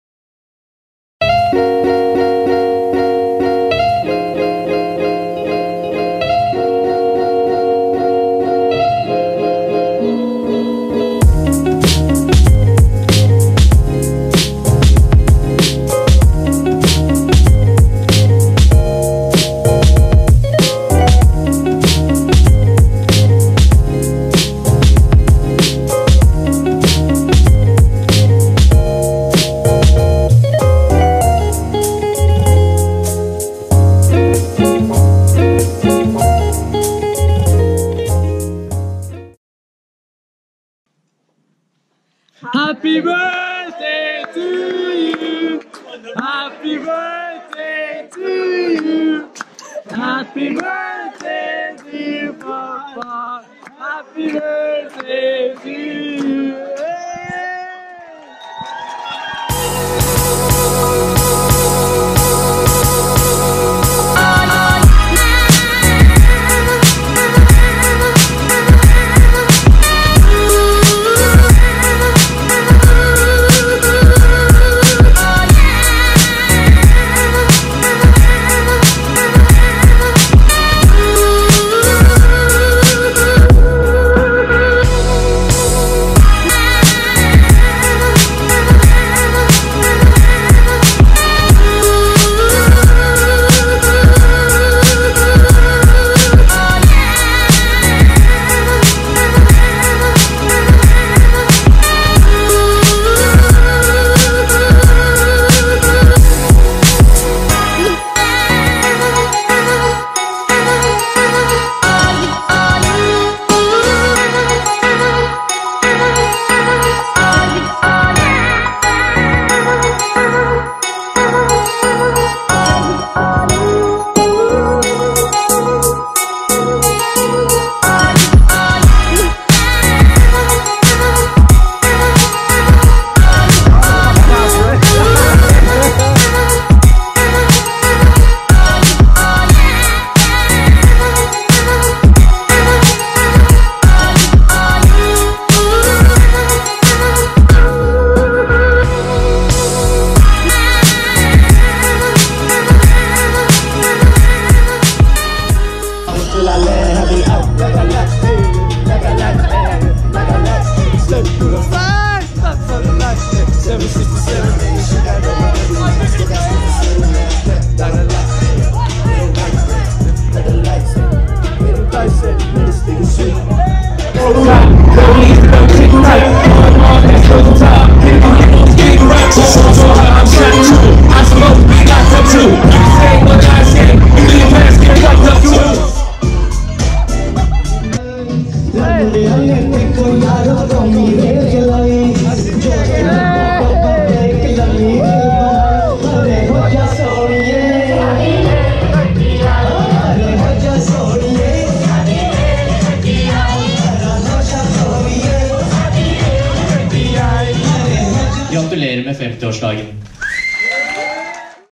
SURPRISING MY DAD FOR HIS 50th BIRTHDAY!
Karan Singh Bhellay - Surprise party for my dad!